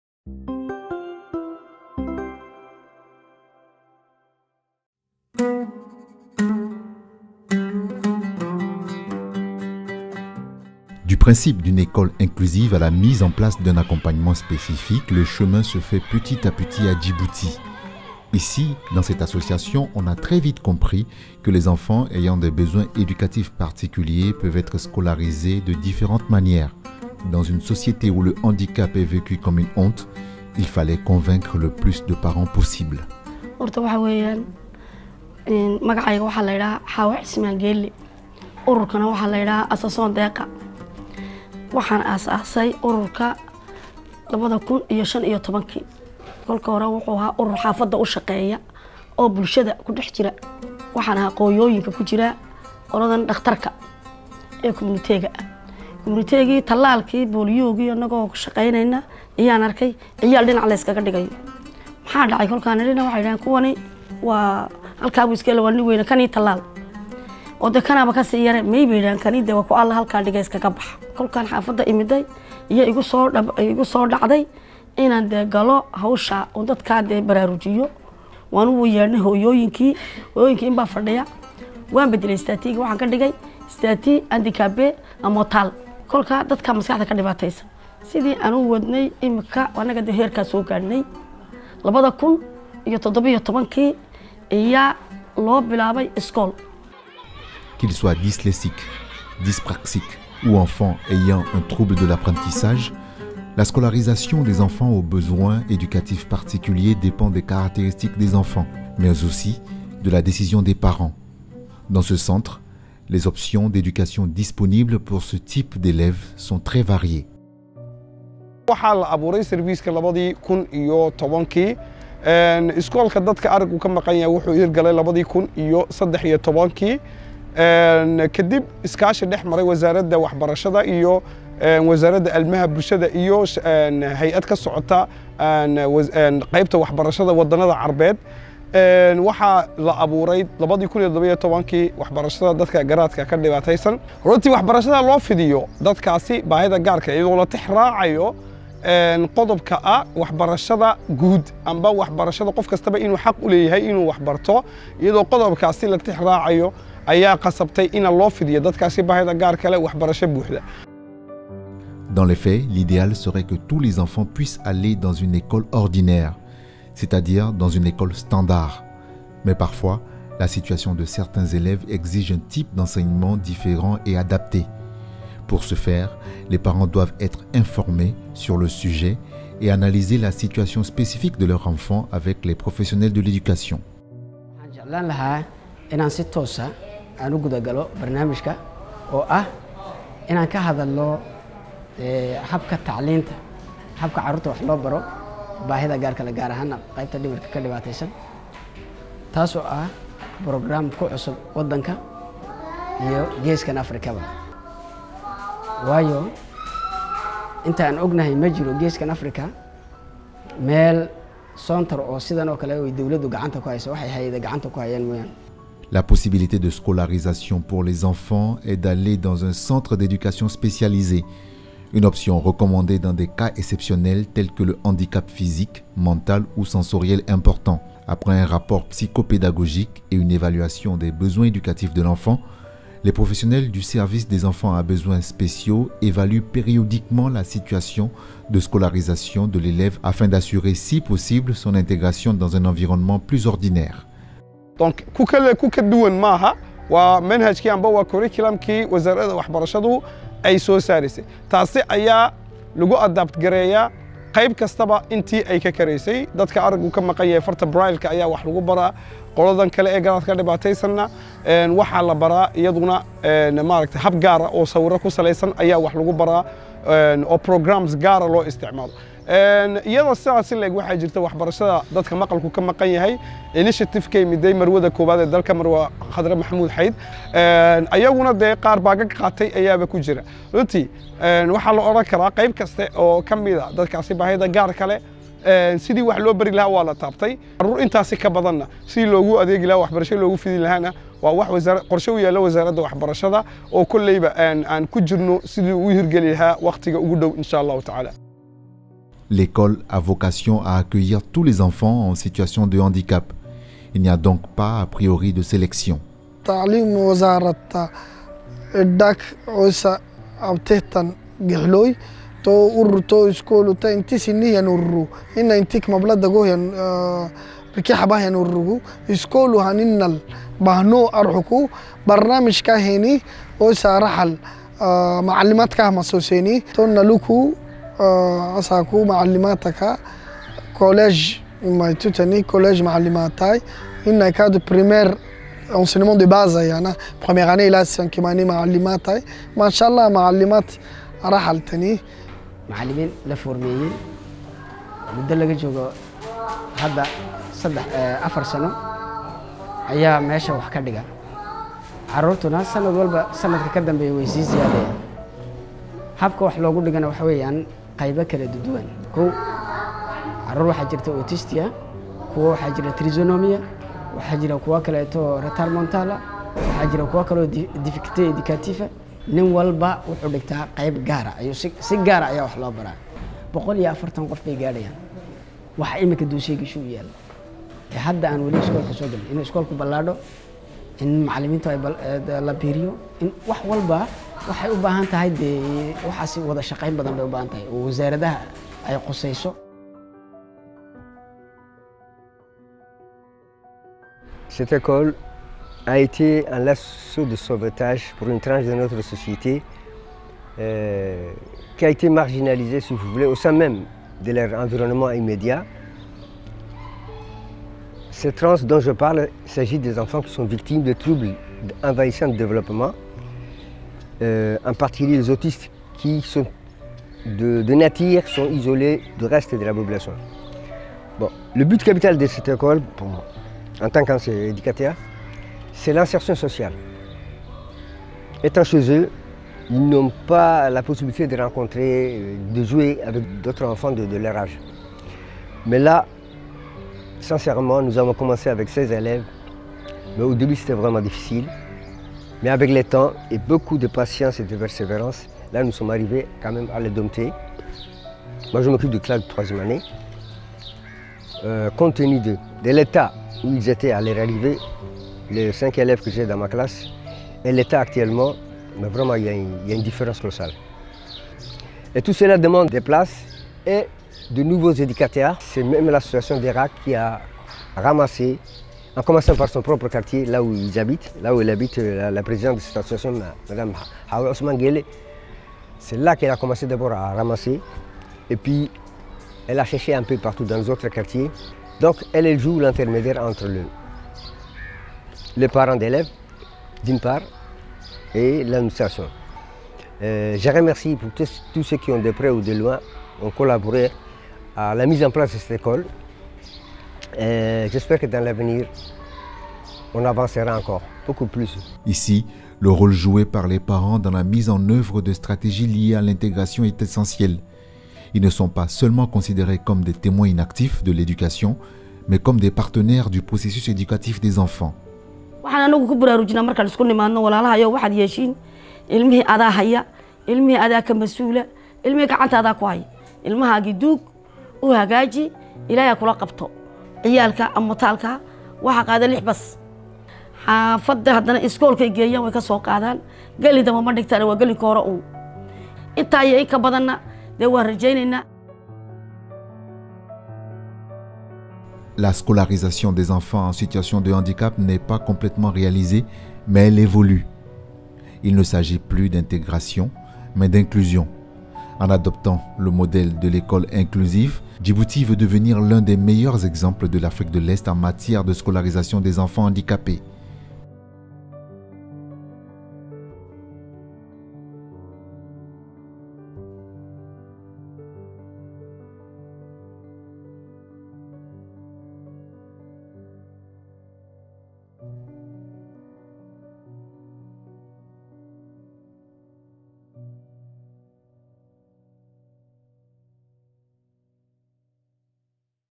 Reportage sur l'école des enfants à besoin spéciaux à Djibouti
Reportage sur l'école des enfants à besoin spéciaux à Djibouti, réalisé dans le cadre de la campagne nationale contre la discrimination et la stigmatisation des personnes handicapées en partenariat avec l'UNICEF Djibouti.